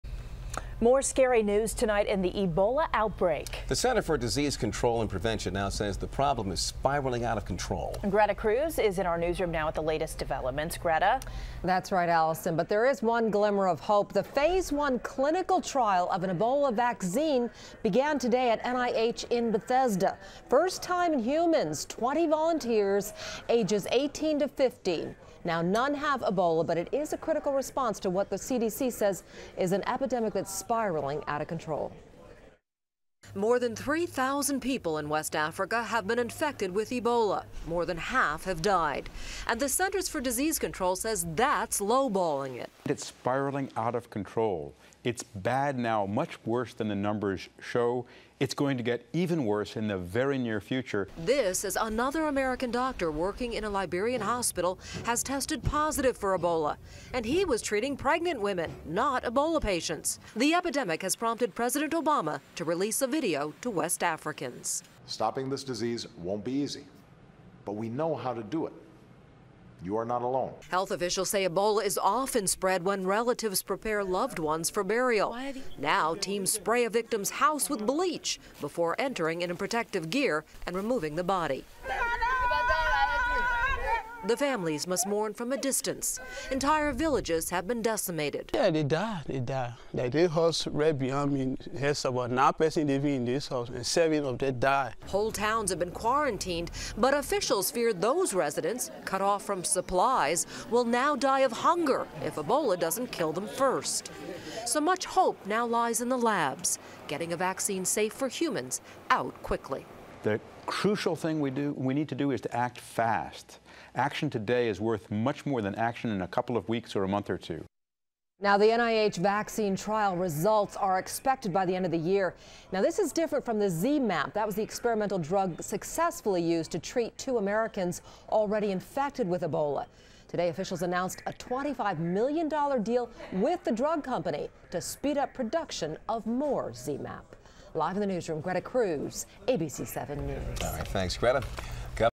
Group says world is losing battle against Ebola
The international group Doctor Without Borders warned Tuesday that the world is losing the battle against Ebola and lamented that treatment centers in West Africa have been "reduced to places where people go to die alone."